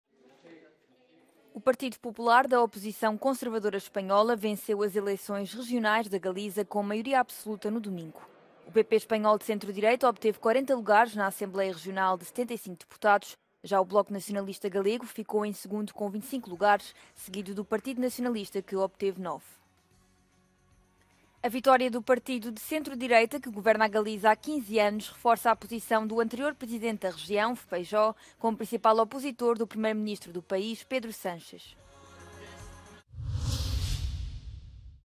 Partido Popular espanhol vence eleições na Galiza com maioria absoluta
O Partido Popular da Galiza venceu as eleições autonómicas de domingo, mantendo a maioria absoluta que detém no parlamento desde 2009.